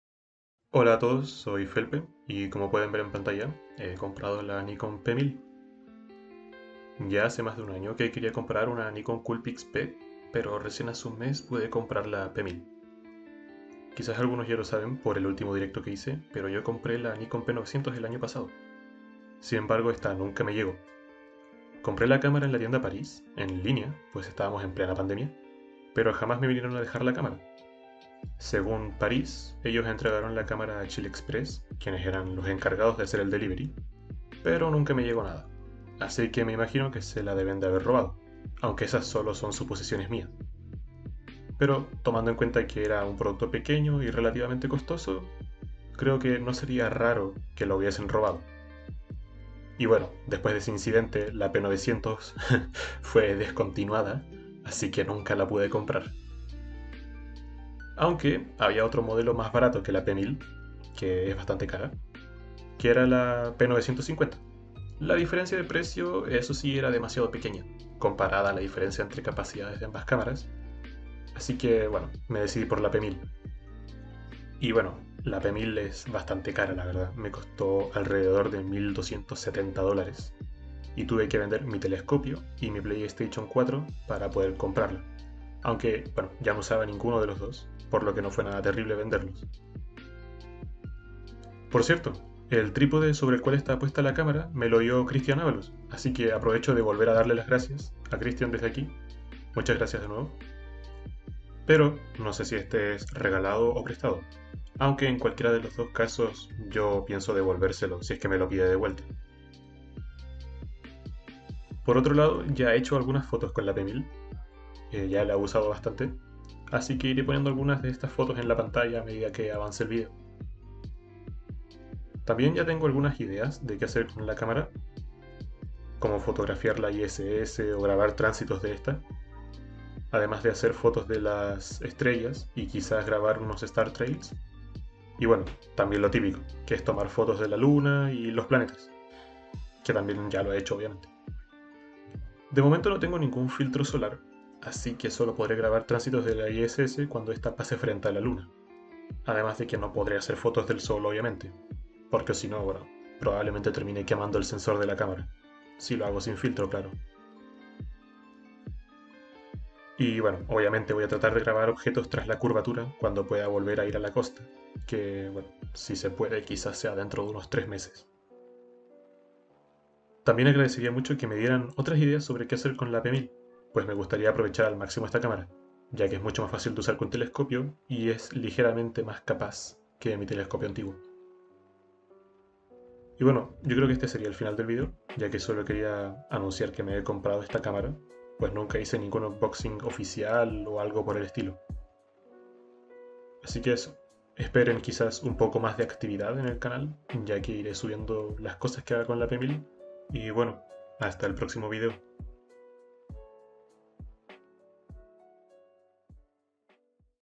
Unboxingn't P1000
Tal y como ven, por fin pude comprar la Nikon P1000, sucesora de la famosa cámara P900.
Idealmente subiré algunos cuantos videos y/o fotos que realice con la P1000, por lo que debería aumentar mi actividad en YouTube, aunque tampoco será mucho.

También agradecería que me diesen algunas ideas sobre que hacer con la cámara, para sí sacarle el máximo provecho que me sea posible.